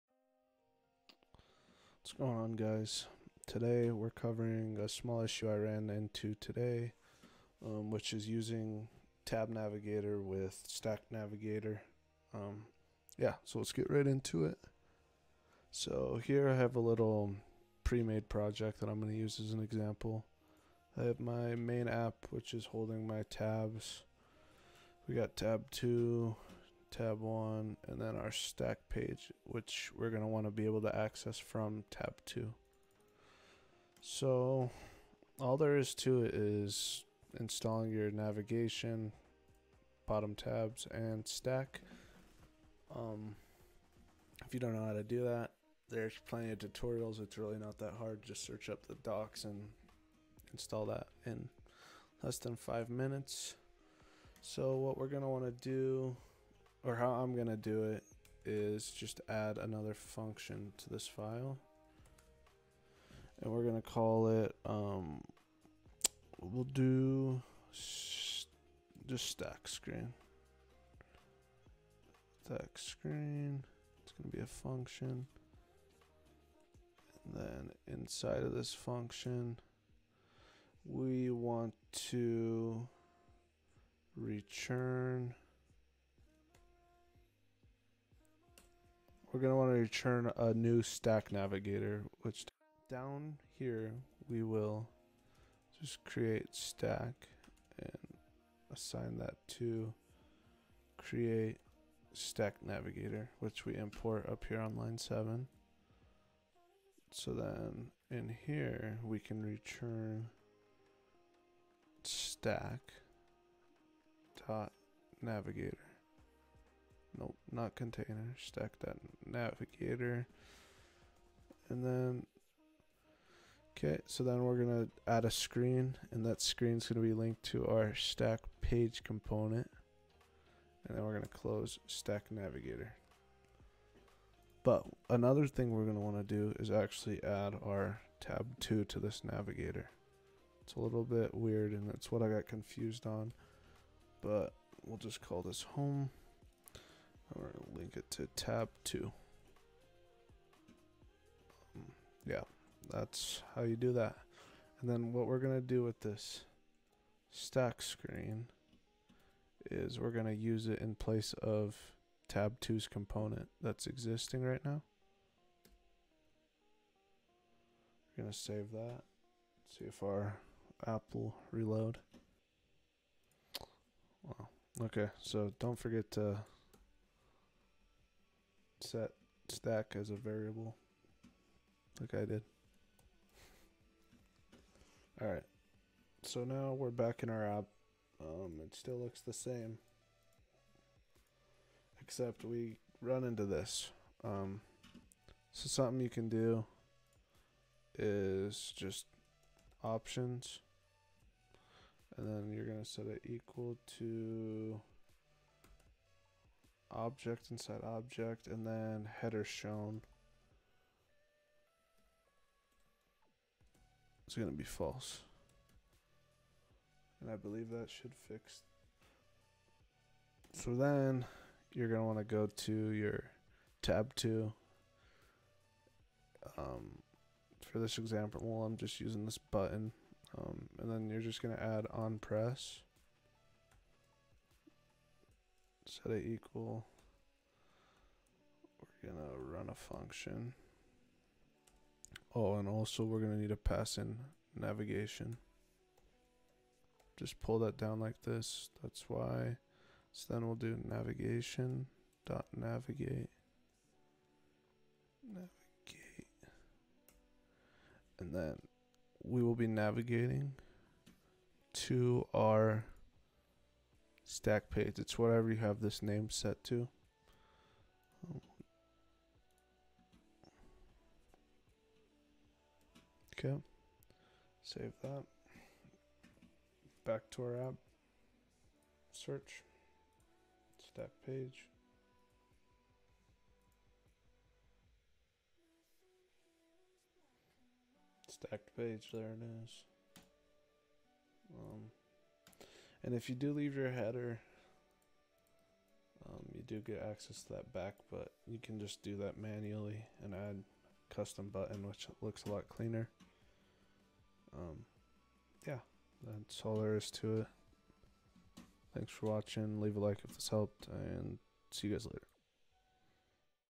React Native: How To Combine React-Navigators (TabNavigator + StackNavigator)
In this video i show how easy it is to combine navigators in react native apps